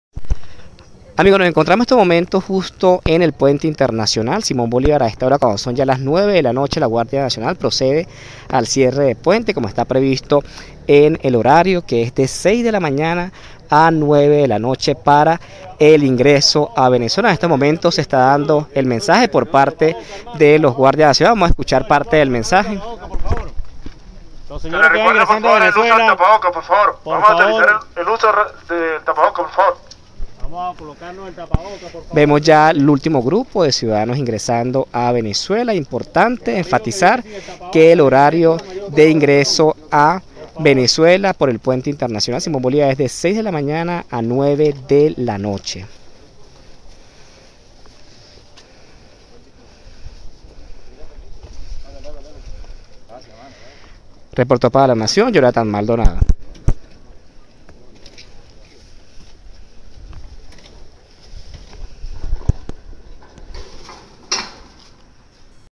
FRONTERA / El ingreso a Venezuela es hasta las 9:00 p.m
Desde que se reactivó el paso peatonal por el puente internacional Simón Bolívar, el pasado 25 de octubre, el ingreso a Venezuela se ha establecido desde las 6:00 a.m. y hasta las 9:00 p.m., hora venezolana.